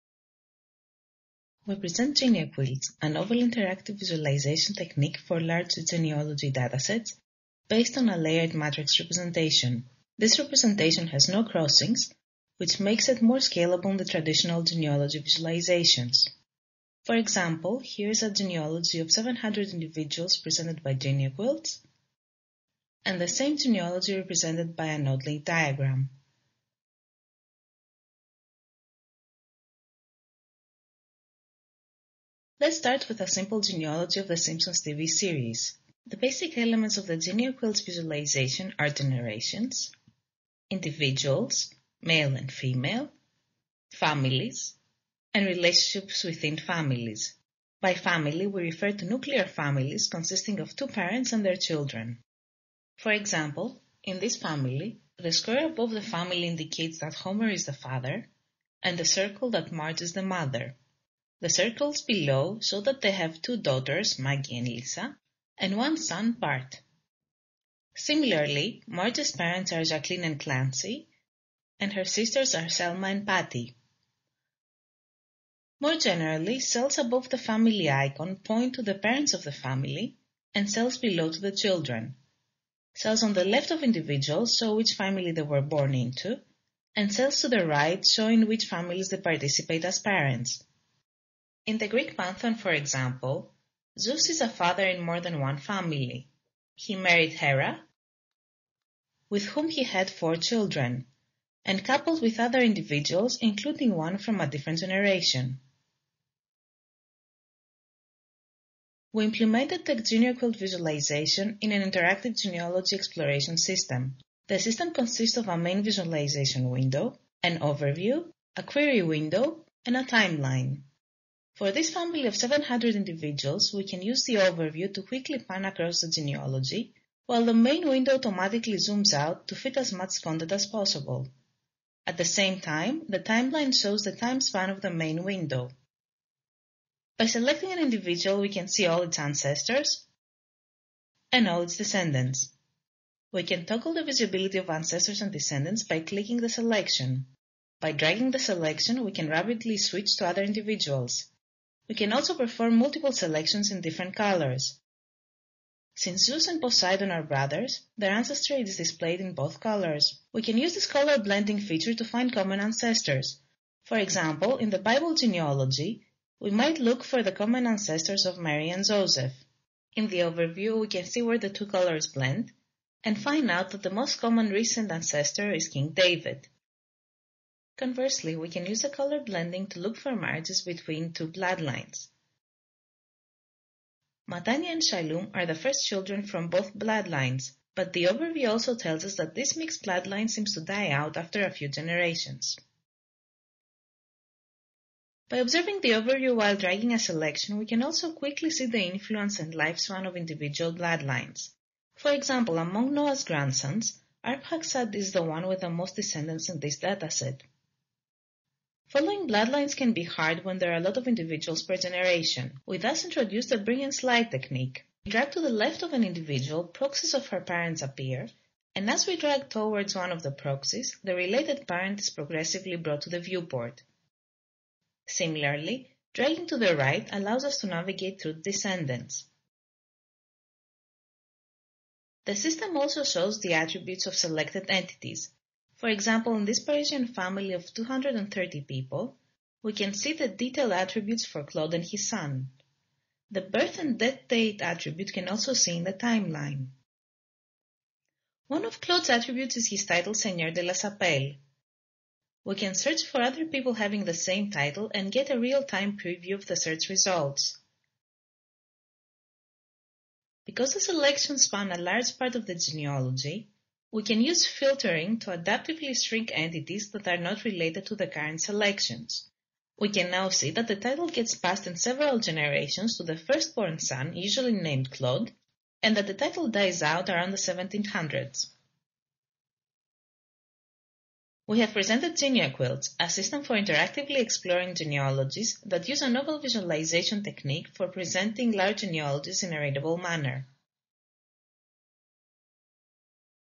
geneaquilts-infovis2010.avi
GeneaQuilts is a new technique for visualizing and interacting with large genealogies, based on a layered matrix representation. It has been implemented as part of a system that presents temporal and spatial overviews, and allows visual exploration, querying and data filtering.
Presented at IEEE InfoVis 2010 by Anastasia Bezerianos, (Pierre Dragicevic, Jean-Daniel Fekete, Juhee Bae and Ben Watson)